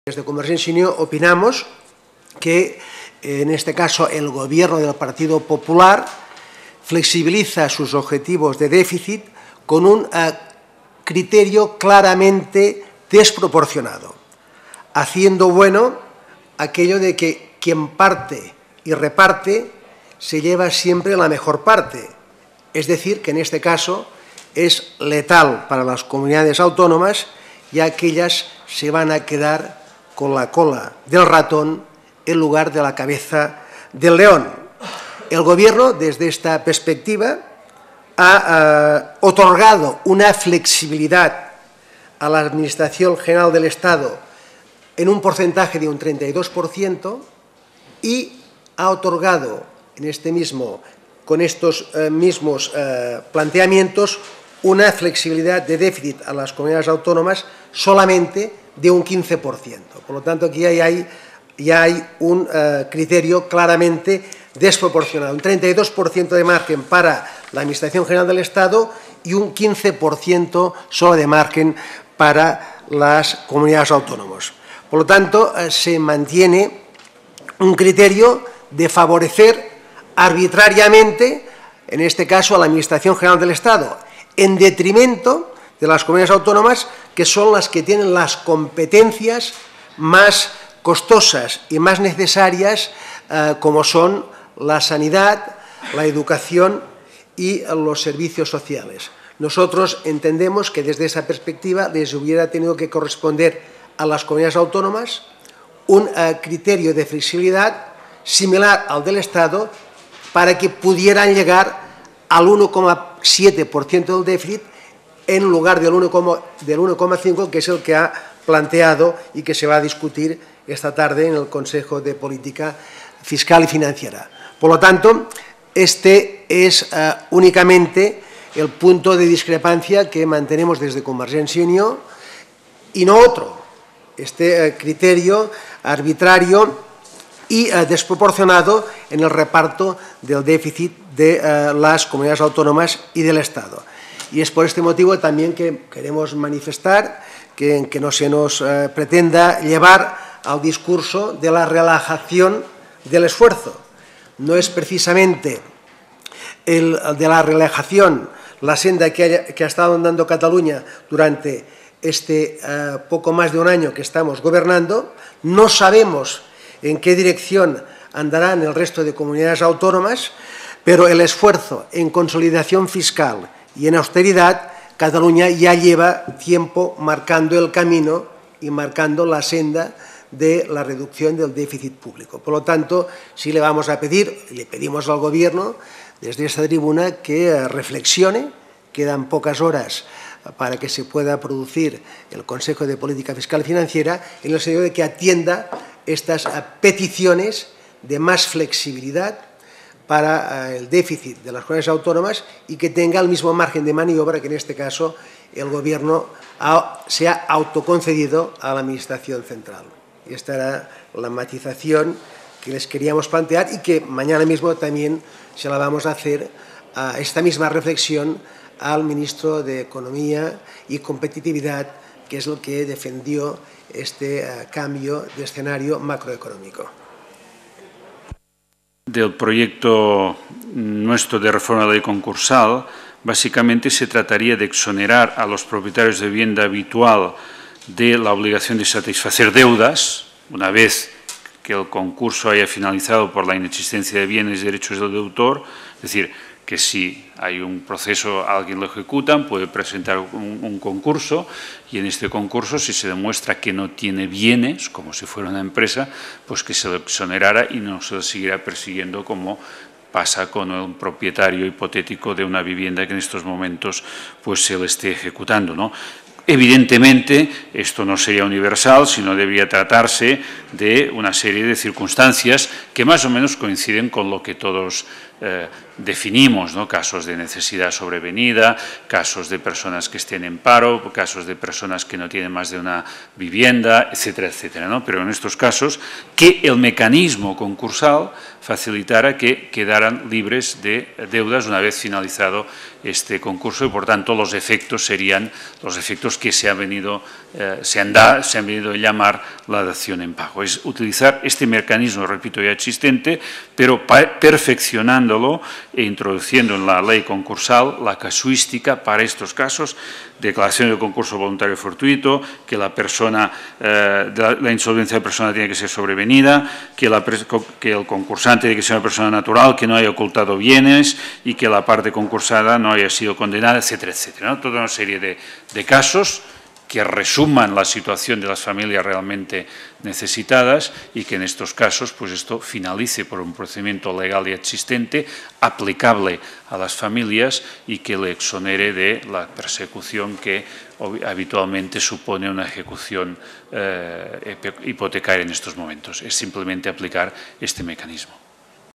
Sánchez Llibre demana al govern de l'Estat que reflexioni i flexibilitzi l'objectiu dèficit CCAA
El Grup Parlamentari Català aposta per reformar la Llei Concursal per fer efectiva la dació en pagament (06/03/2012)